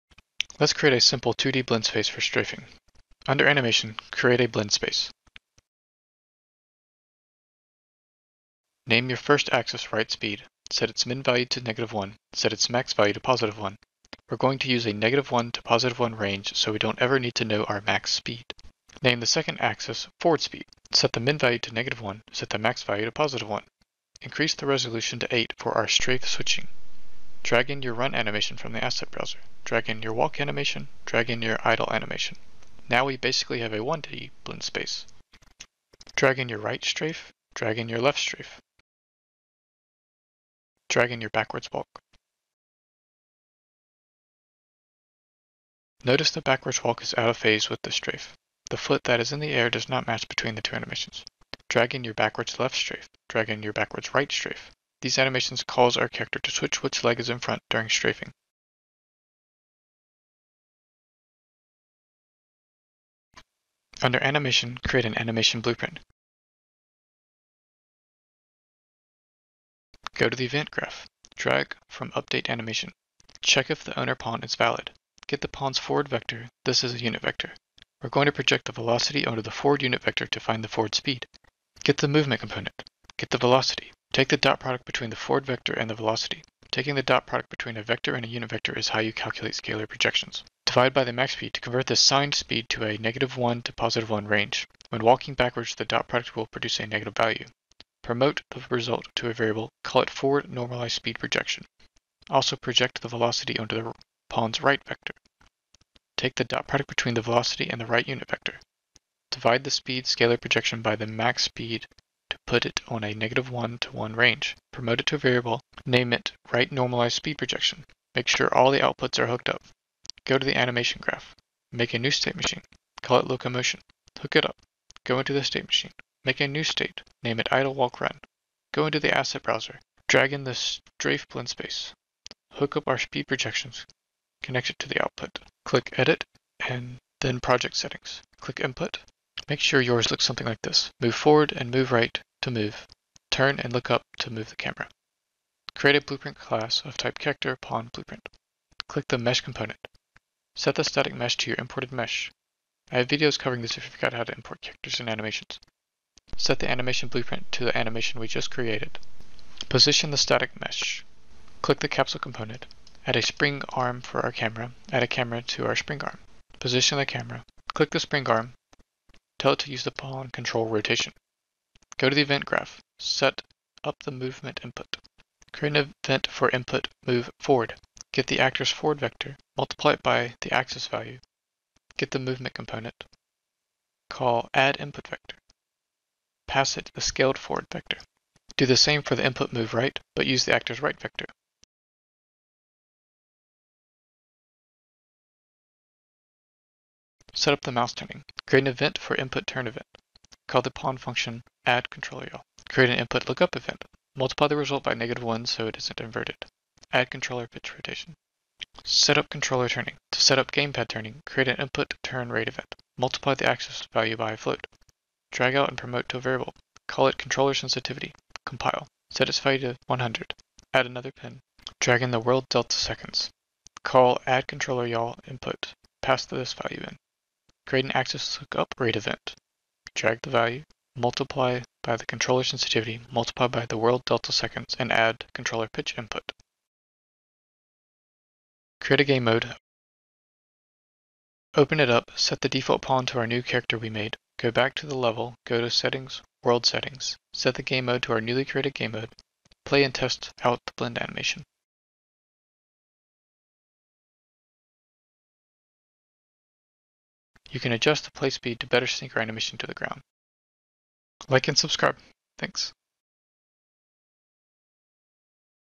Quick Reference: Unreal using Blendspace 2D for simple strafing movement
Note, I think that in order for the animations to blend correctly, all of them should have aligned keyframes. (eg all are 30 frames, with middle at frame 15, etc).

This is intended to be a memory jogging reference, not necessarily a tutorial. I may make a tutorial explaining these concepts one day when I've better mastered them.

There are certainly other ways to do this.

0:00 Creating The Blend Space
0:07 Setting Up The Horizontal and Vertical Axes
0:27 Adding Animations To The Blend Space
1:01 Creating Animation Blueprint (Animation Instance)
1:09 Anim BP - Event Blueprint Update Animation - Speed Projections
1:59 Anim BP - State Machine - Set Up Locomotion
2:14 Set Up Input
2:24 Character (Pawn) BP - Set Up Components
2:50 Character (Pawn) BP - Event Graph (Move and Look)
3:58 GameModeBase Set Up
4:09 Test Blending
4:16 Syncing Run Animation To Ground